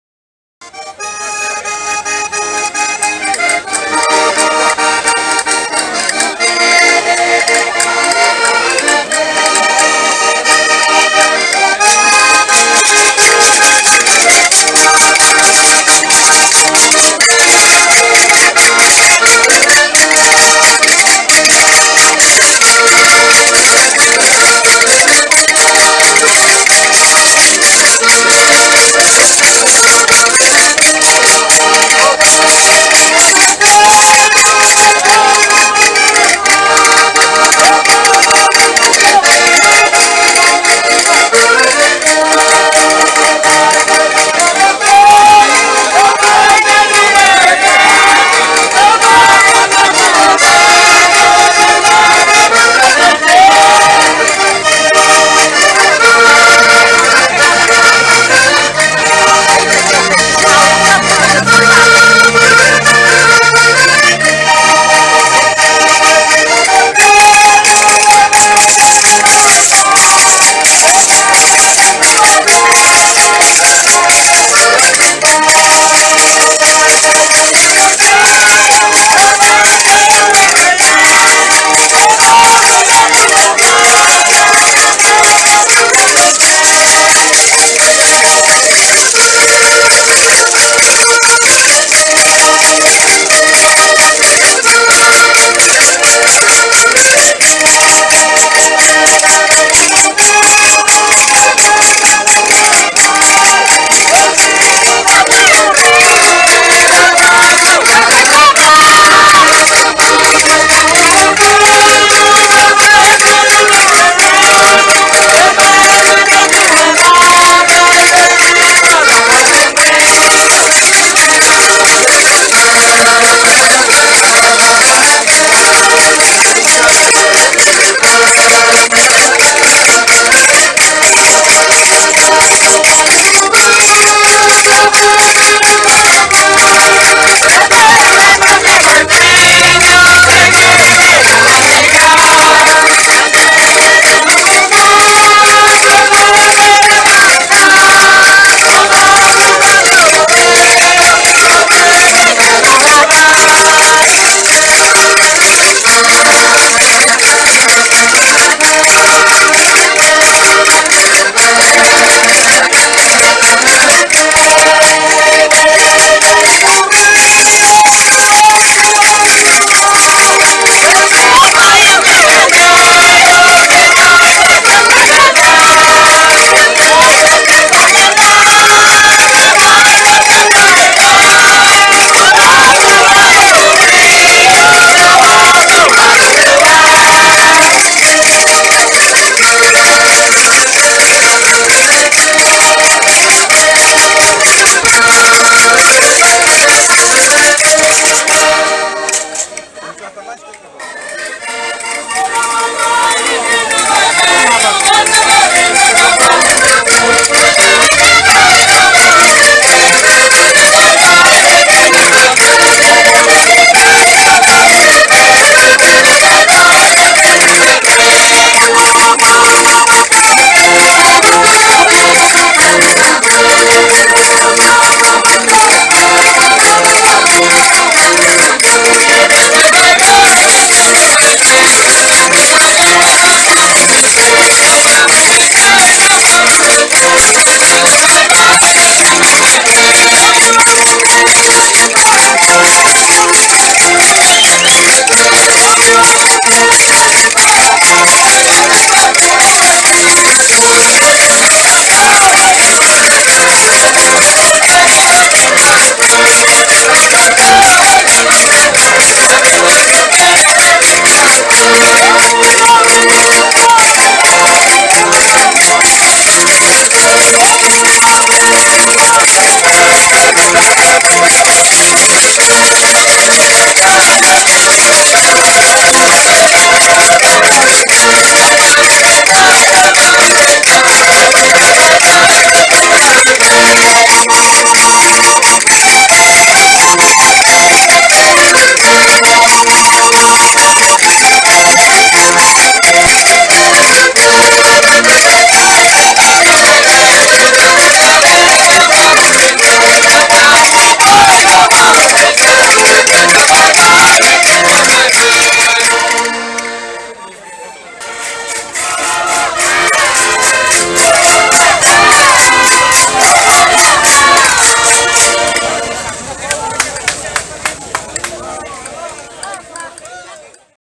Concertinas, Danças e Cantares em ❤️ Arcos de Valdevez ❤️ - 17 de Junho 2018
Um pequeno excerto das Concertinas, Danças e Cantares no Campo do Trasladário, em ❤️ Arcos de Valdevez ❤️ pelo final da tarde de 17 de Junho de 2018